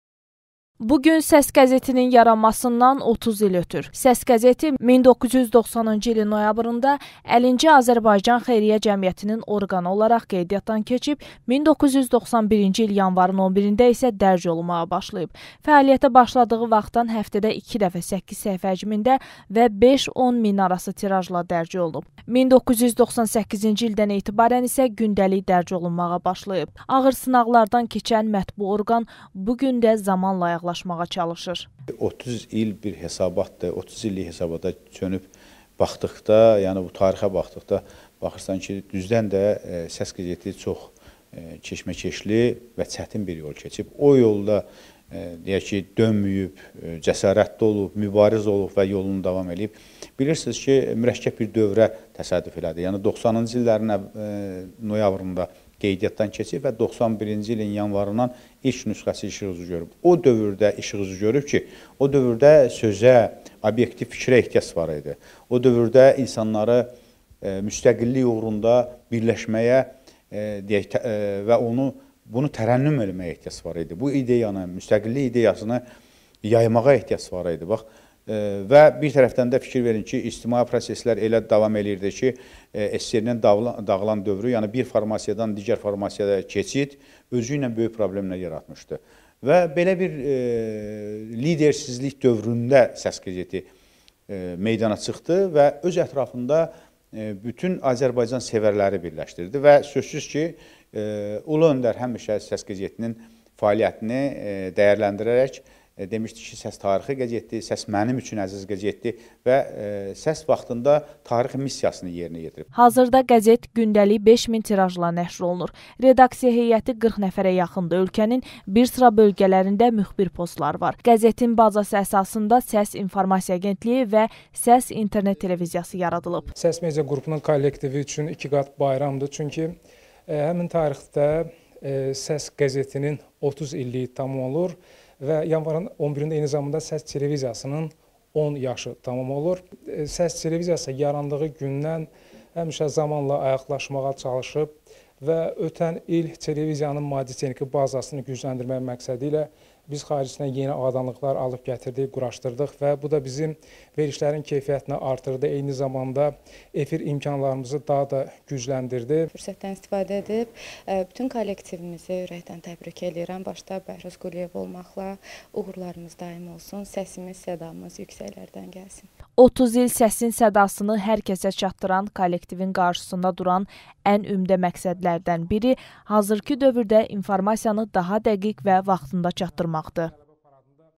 “Səs” qəzetinin yaranmasından 30 il ötür
1991-ci ilin yanvarından dərc olunan qəzetin 2021-ci ildəki uğurları